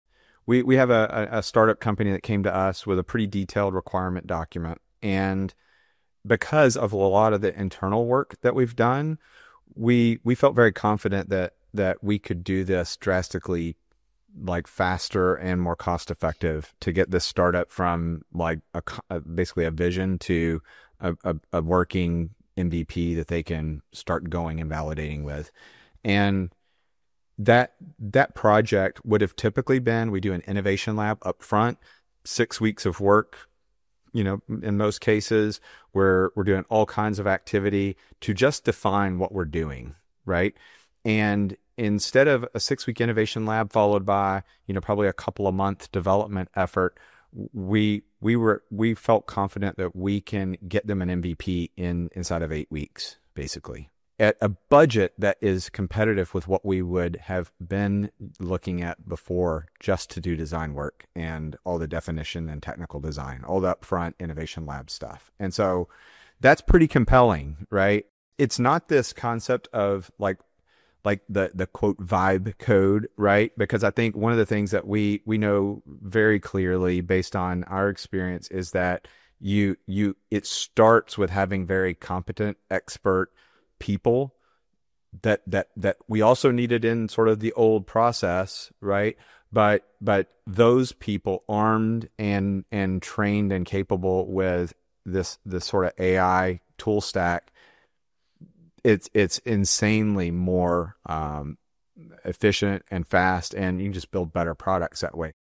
This Used To Take 18 Weeks. AI Helps Us Do It In 8
AI doesn't help every stage of the product journey equally. From nothing → prototype is where you get the greatest lift.

Going from an idea to something you can put in front of users can happen in days instead of weeks thanks to AI-powered design.

We recently turned a startup's detailed requirements doc into a working MVP. This used to be an 18 week process; we did it in 8 weeks.